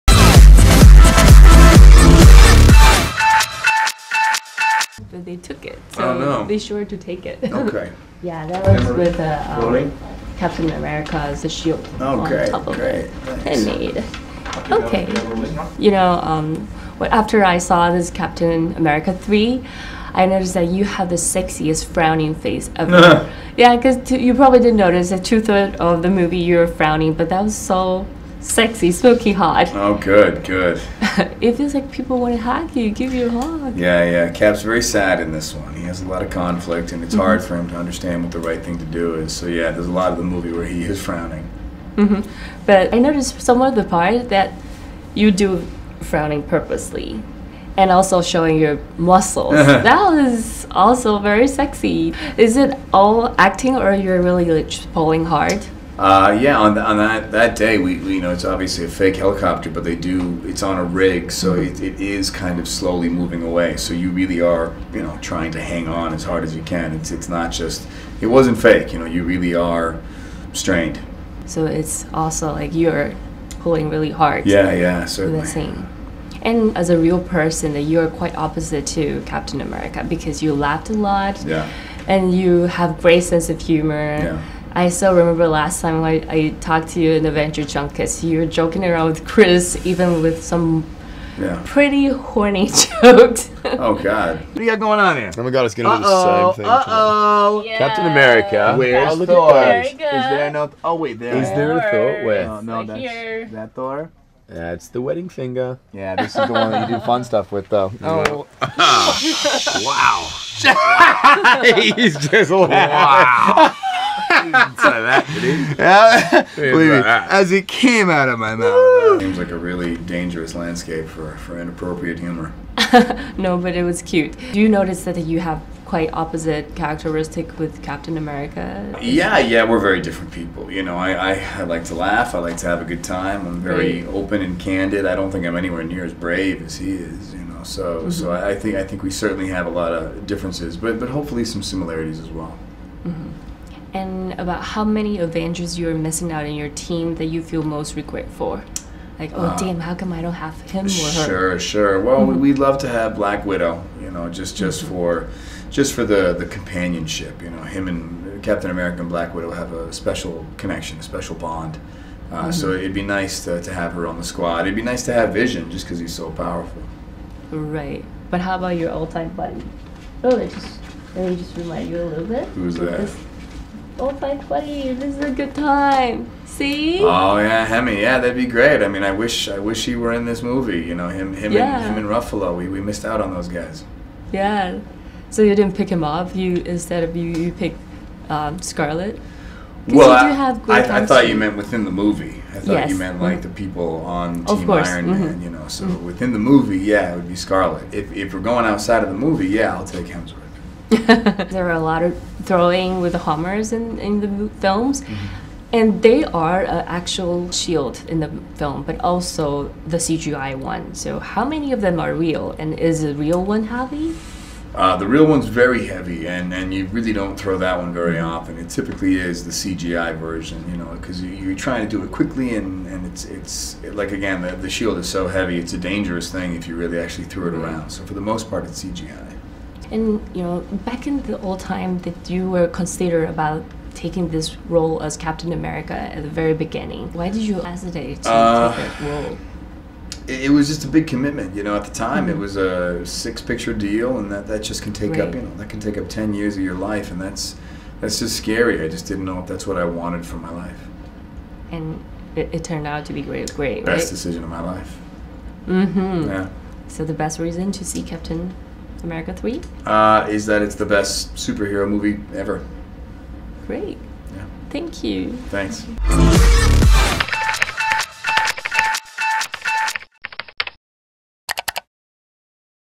專訪《美國隊長3》克里斯伊凡自爆寧拋棄黑寡婦選擇雷神(驚)【電癮好選喆Top movie picks】
登愣～美國隊長3專訪來囉！這次專訪跟上次反差很大，克里斯伊凡根本就入戲太深！連專訪時也一直皺眉頭...(已被帥暈)
但聊到電影問題時，又滔滔不絕說出自己看法，更自爆可以選擇的話，寧願選擇雷神克里斯漢斯沃來跟自己同一陣線，頓時讓立喆瞬間噴鼻血XDD(猛塞衛生紙)。
別忘了訂閱【電癮好選喆Top movie picks】頻道&簡立喆主播臉書 按個讚，最新訊息不漏接!